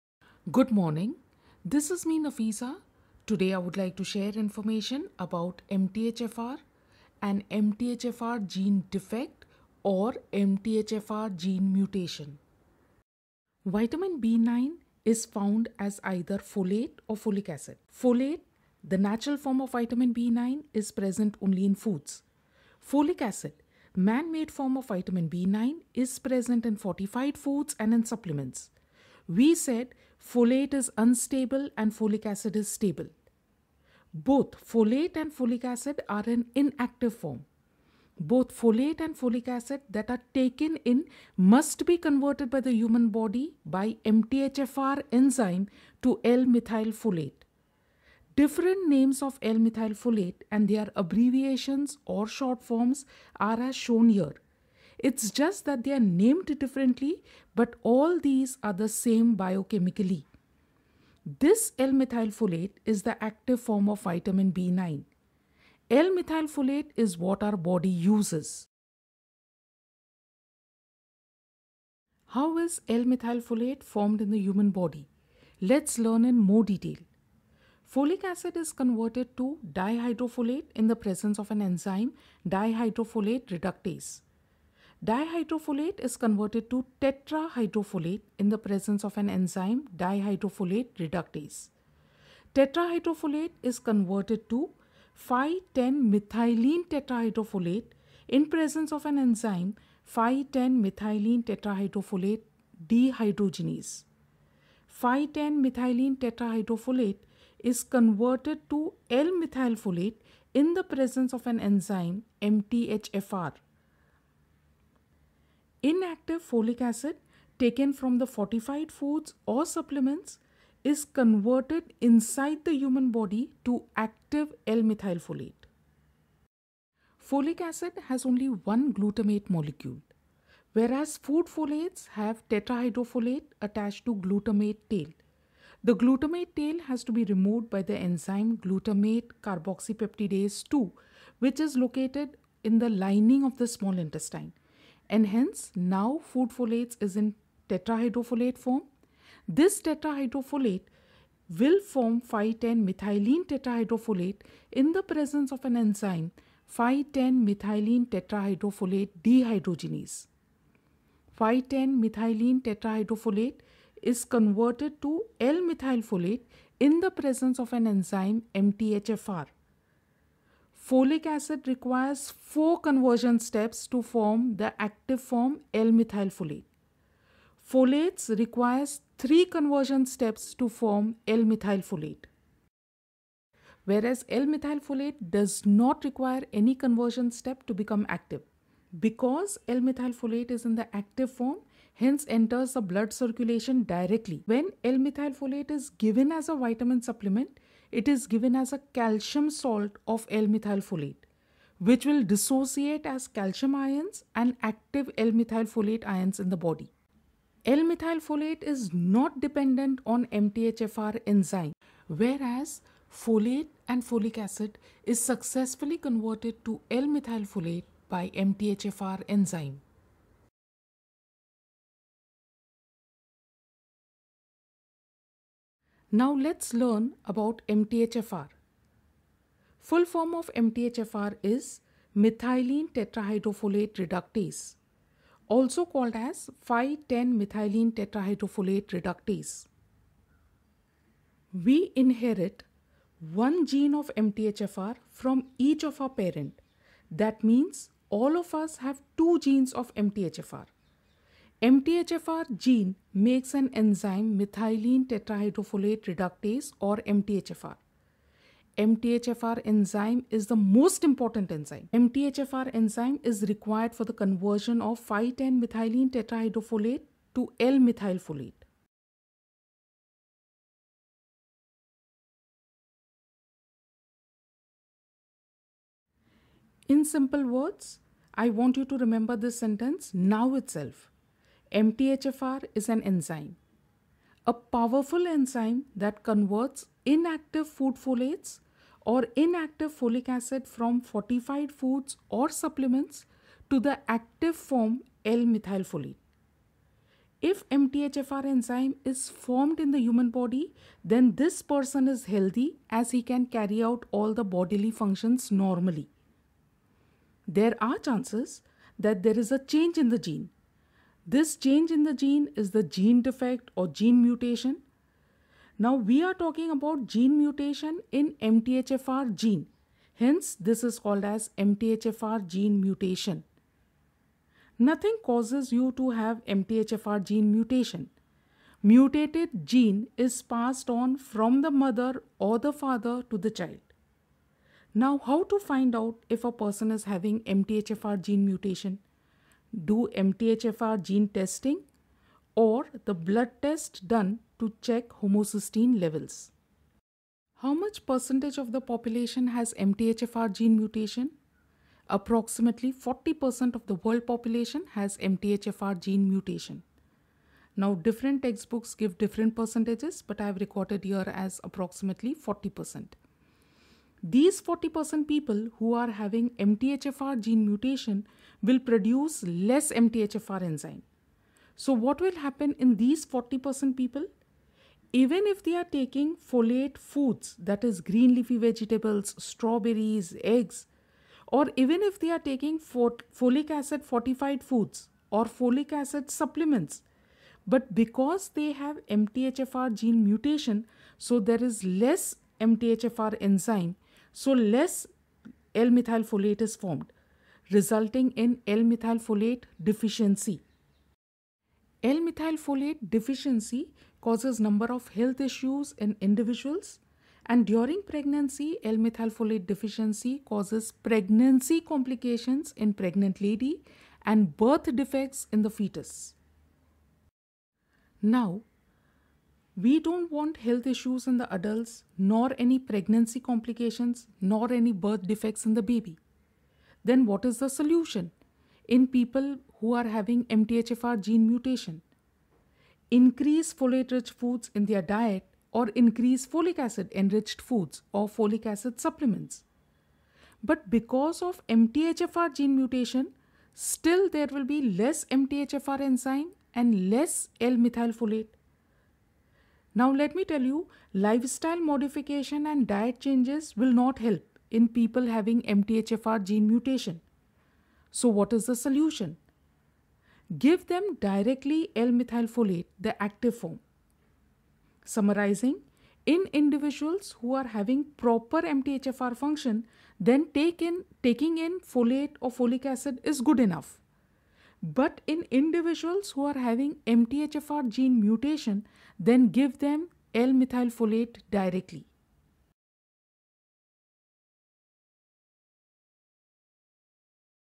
MTHFR (23Jan23)
methylenetetrahydrofolate reductase
5,10-methylenetetrahydrofolate reductase

MTHFR GENE DEFECT
MTHFR GENE MUTATION

what is mthfr
why mthfr gene mutation
mthfr gene mutation explained

Both Folate & FA that are  taken in are INACTIVE FORMS and MUST BE converted in human body  by MTHFR ENZYME to L-methylfolate, active form of Vitamin B9

00:00:00 Introduction
00:00:12 MTHFR converts Folates & Folic acid to L-methylfolate
00:01:12  MTHFR converts Inactive Folic acid to L-methylfolate
00:02:05  MTHFR converts Inactive Folates to L-methylfolate
00:03:03 L-methylfolate requires NO conversion step to become ACTIVE
00:03:26 L-methylfolate is not dependent on MTHFR enzyme
00:03:55 What is MTHFR
00:04:57  MTHFR if formed in human body – Healthy
00:05:07 What is MTHFR GENE DEFECT OR MTHFR GENE MUTATION
00:05:34 How to find out if a person is having MTHFR GENE MUTATION
00:05:46 Percentage of population having  MTHFR gene mutation
00:06:11 what happens in 40% people who are having MTHFR gene mutation
00:06:58  Solution  in people who are having MTHFR GENE MUTATION
00:07:44 Summarizing